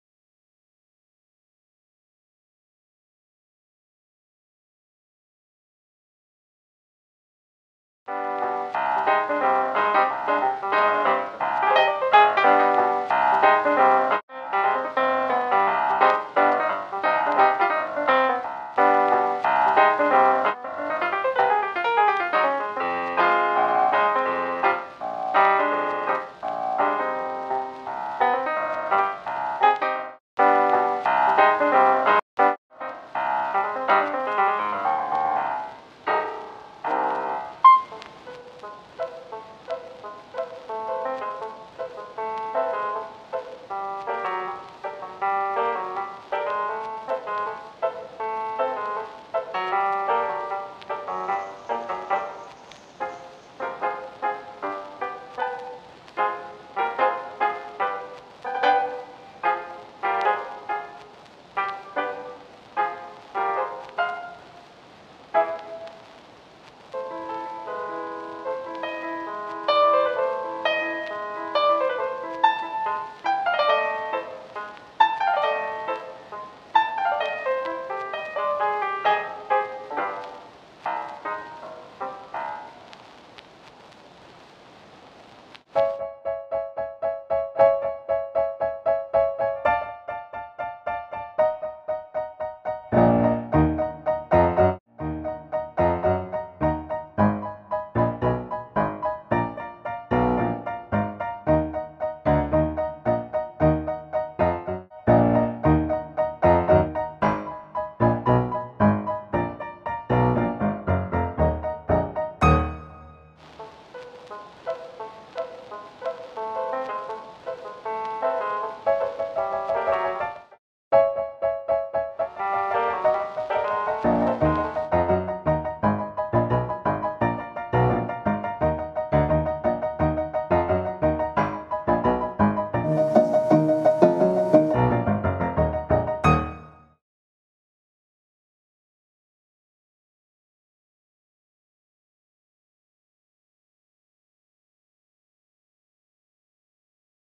The Great Defeat of Dr. Duno Good [a silent movie]
A silent movie made by Kym Warner, Maggie Eckroad, Janae Davidson, Haley Heilmen, Tabby Mckinney, Joe Costello, and Corey Hall!